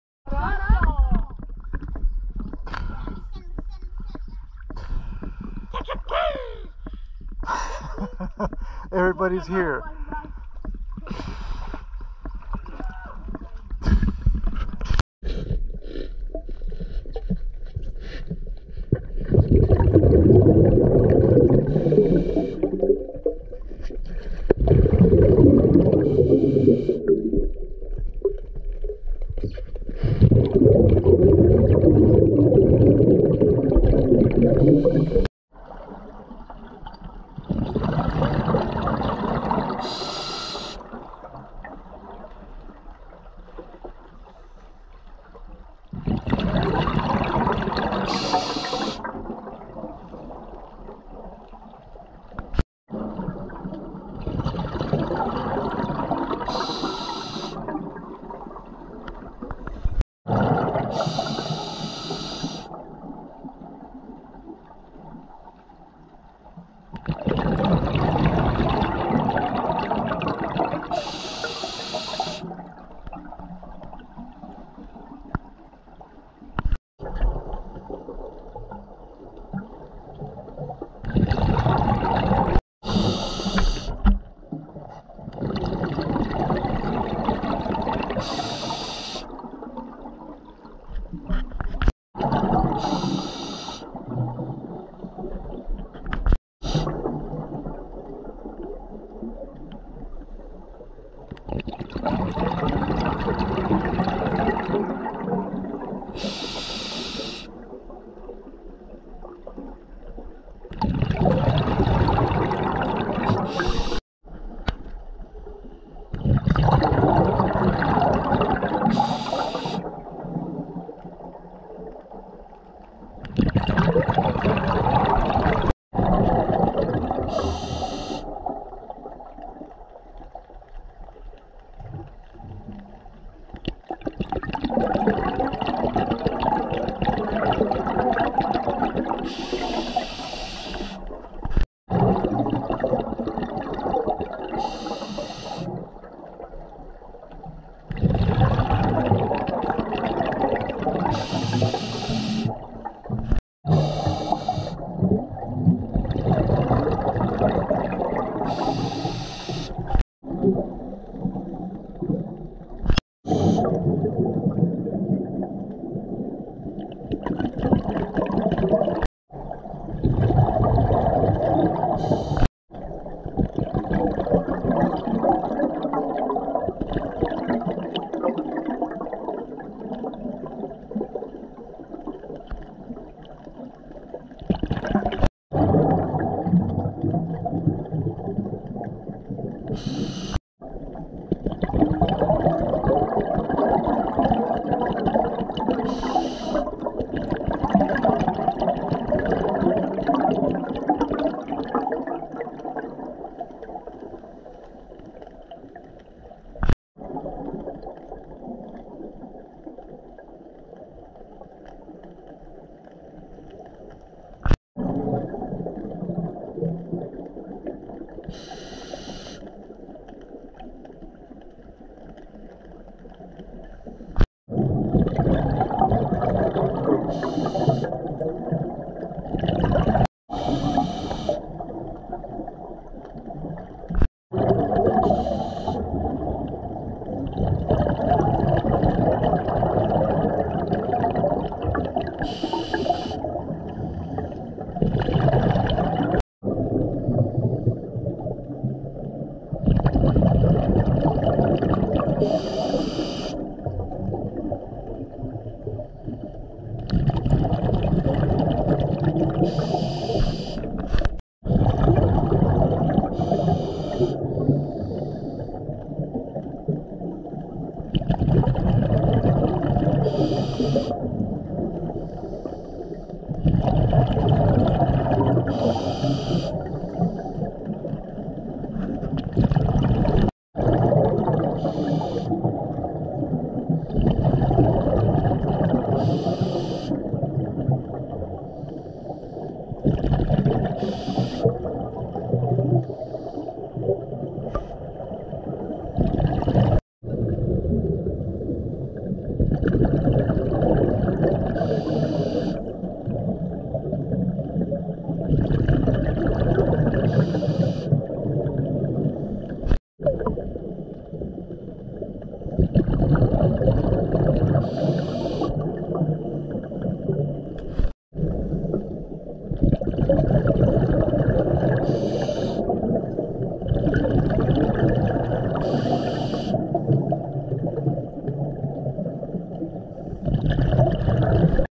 Lions Cove FULL
Video from our incredible dive at Lions Cove, Lake Malawi.